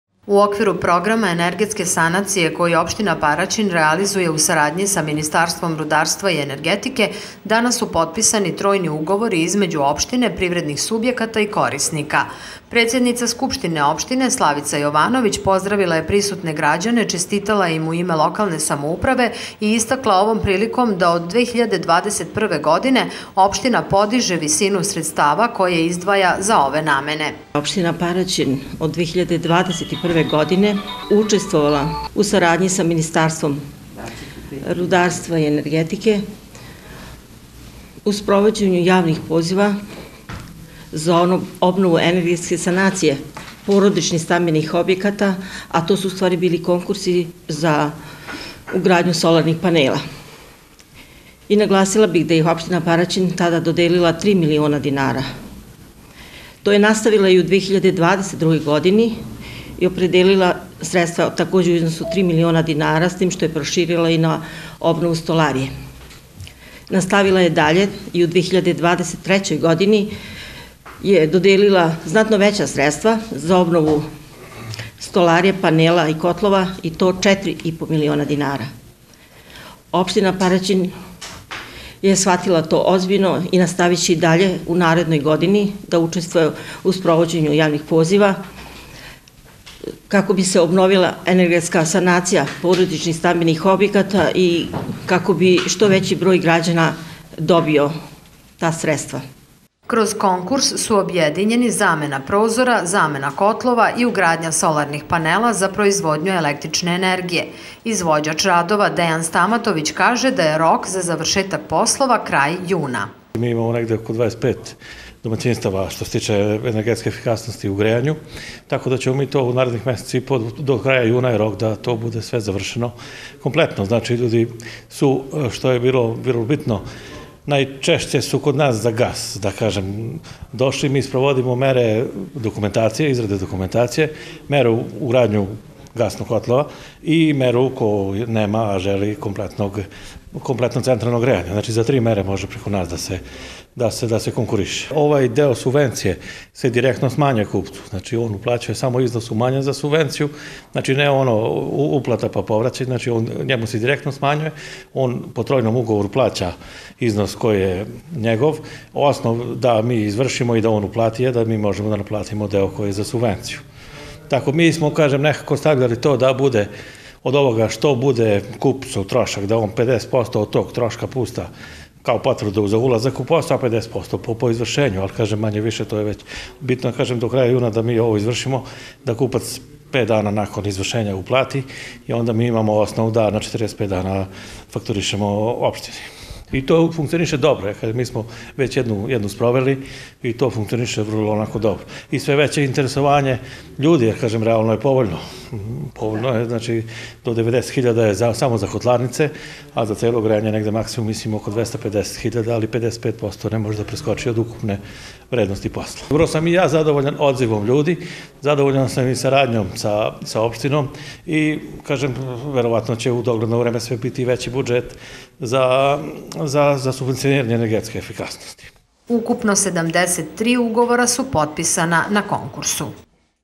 Потписани уговори за енергетску санацију
Општина Параћин и Министарство
рударства и енергетике
суфинансирају Програм енергетске
санације породичних кућа и станова где
је 40 процената удео Општине а 60
Министарства. То практично значи да је
Општина Параћин са 4 и по милиона
динара помогла корисницима да путем
остваривања права на Конкурсу, обаве
енергетску санацију у својим
домаћинствима, а реч је о замени
односно уградњи котлова, соларних
панела и столарије. Укупно 73 уговора
су потписана у оквиру наведеног
конкурса.